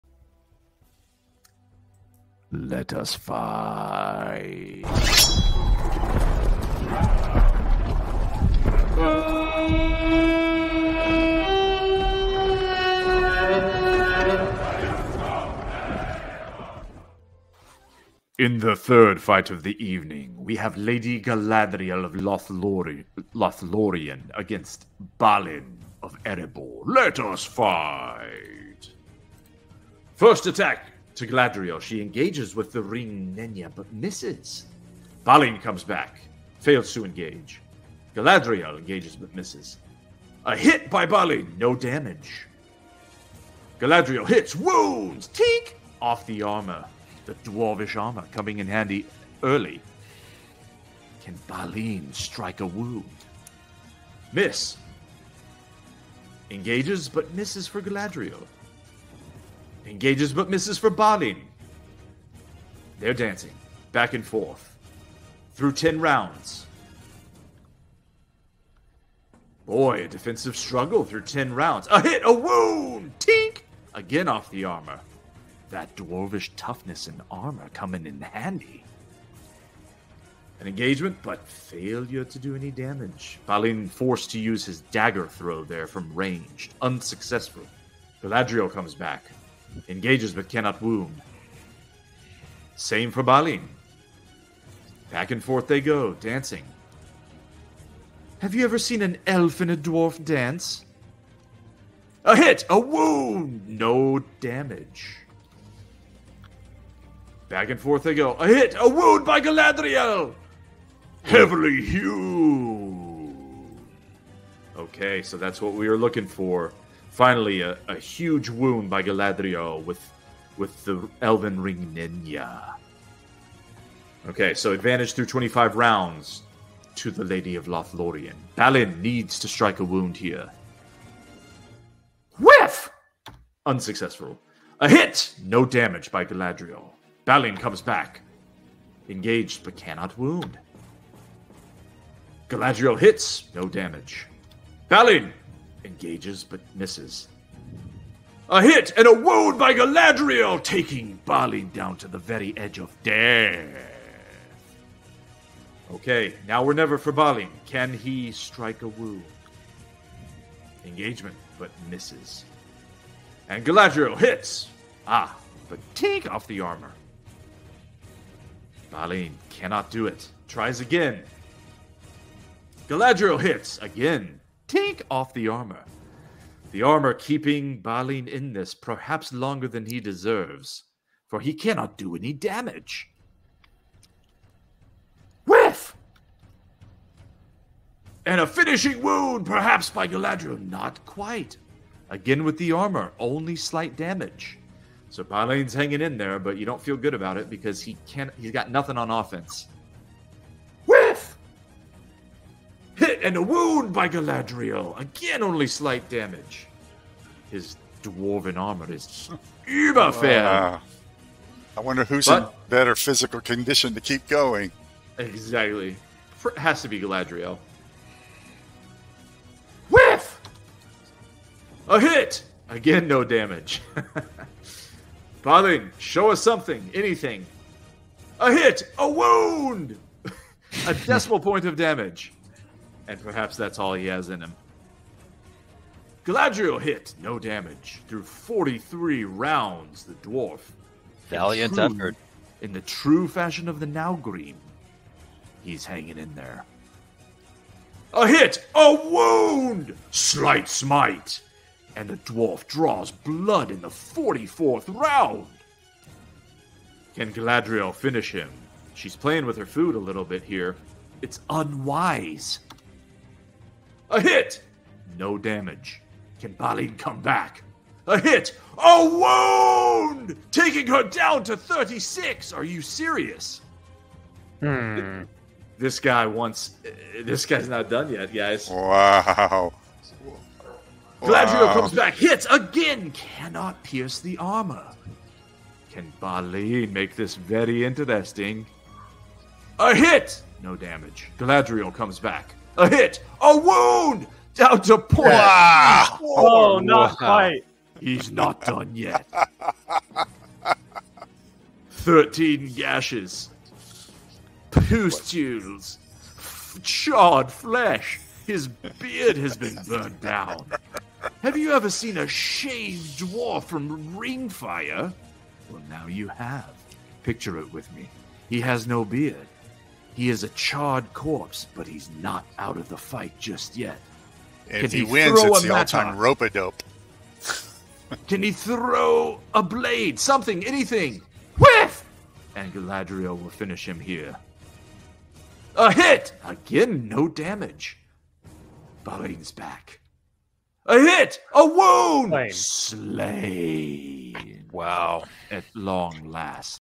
3. Galadriel vs 8. Balin
Third Age Throwdown - Second Round - The victorious challenger, Balin, advances to the 2nd round where he faces The Lady of Lothlorien.